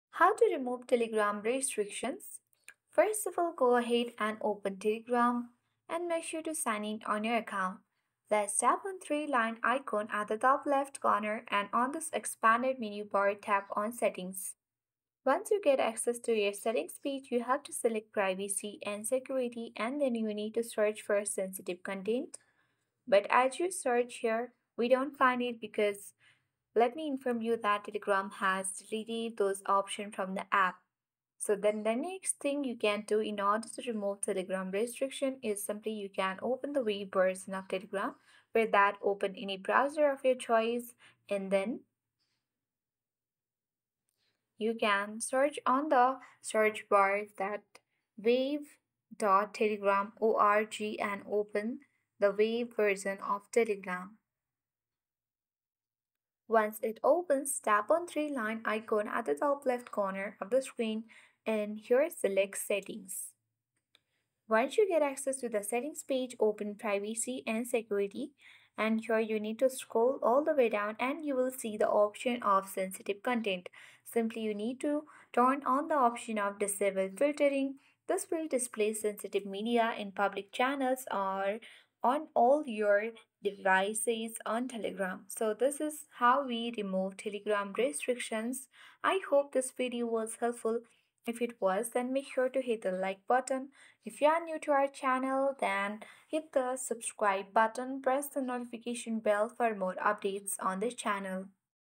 How To Remove Telegram Restrictions!
Learn how to easily remove Telegram restrictions on your account or group, whether you're using Android or iPhone, in this step-by-step tutorial. Overcome content and forwarding limitations with our expert guidance!

In this video, you'll discover how to remove Telegram restrictions on Android and iPhone. You'll also learn how to manage and remove content restrictions on Telegram and understand how to handle Telegram forward restrictions.

Timestamps:
0:00 Introduction
0:20 Step 1: Accessing Your Telegram Settings
0:45 Step 2: Adjusting Restriction Settings
1:15 Step 3: Verifying Changes